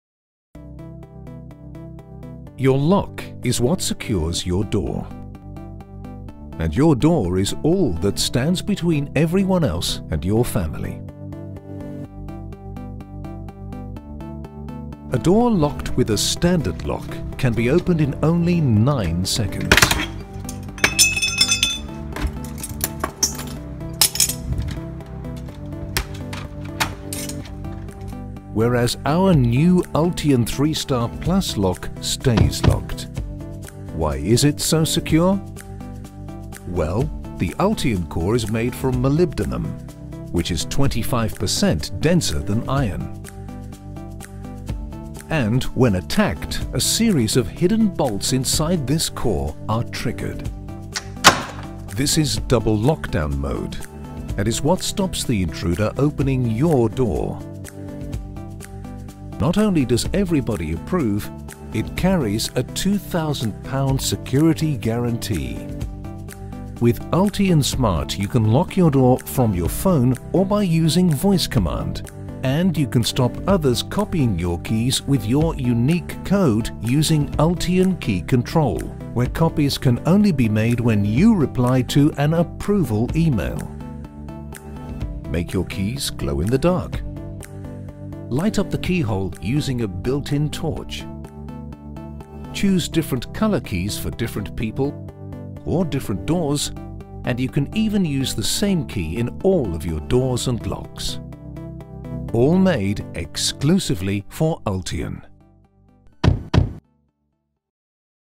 TS007 3 Star & Sold Secure Diamond Grade, Lock, Ultion
Ultion review

This video features Ultion & Ultion Smart two of Help Locks flagship products.

To enquire about purchasing Ultion or if you are in West Yorkshire, having one of the Ultion products supplied & fitted, call